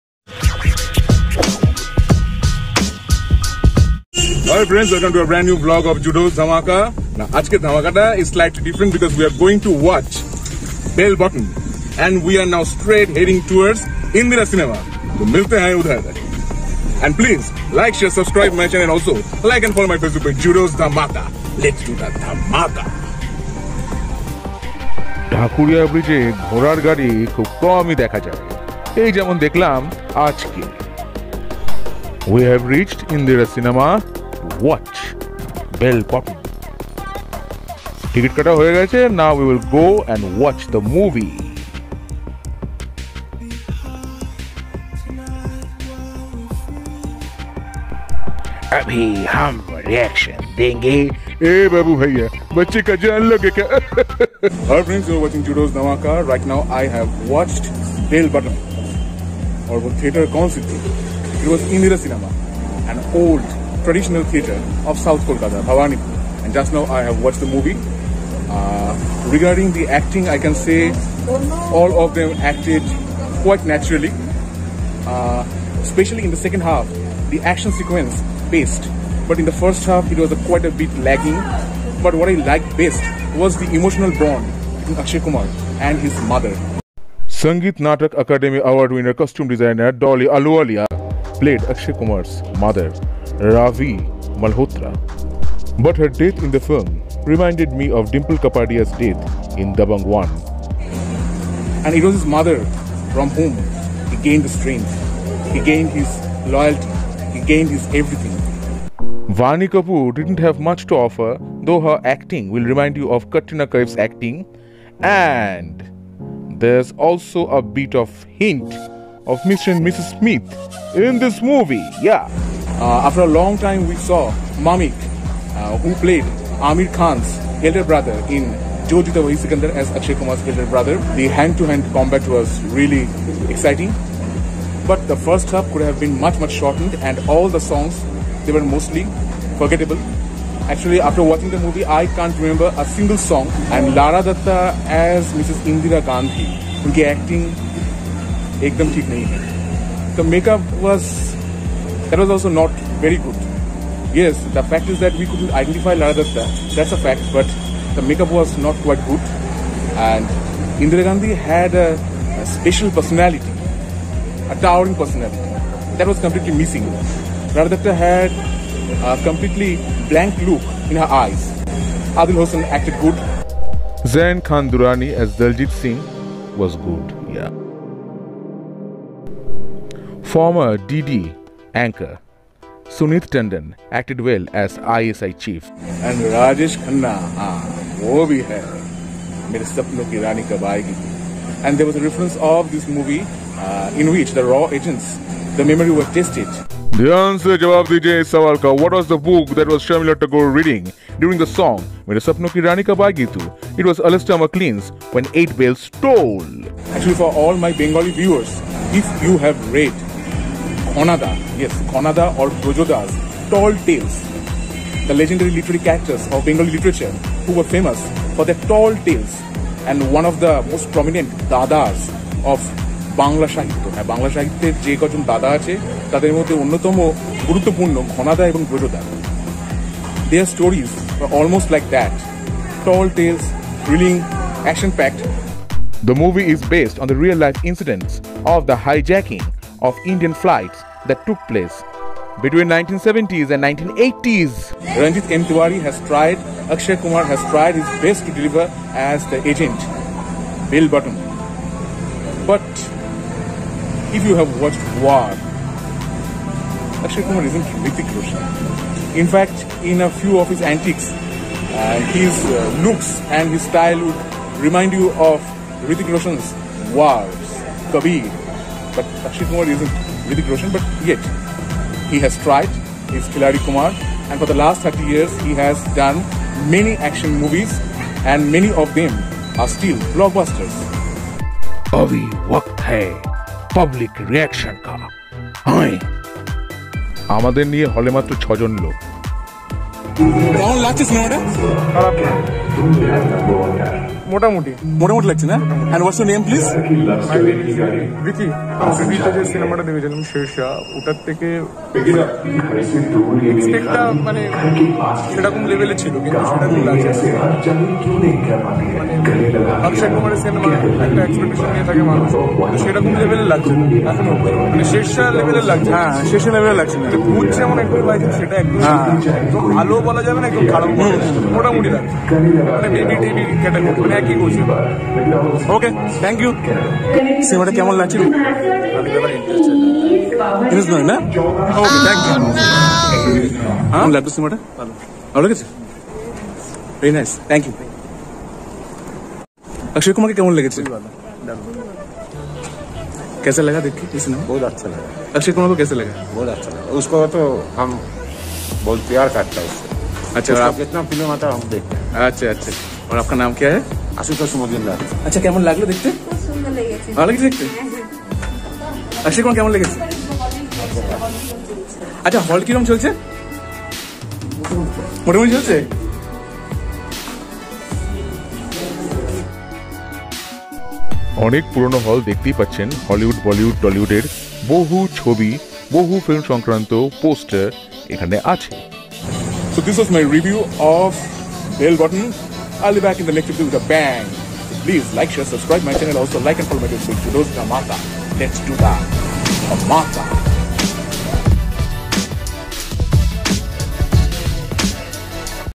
Bell Bottom Review + Public Reaction | Akshay Kumar | Judo' Dhamaka
BELL BOTTOM – Review + Public Reaction!

Vlog By Judo(Sankha Subhra Chatterjee)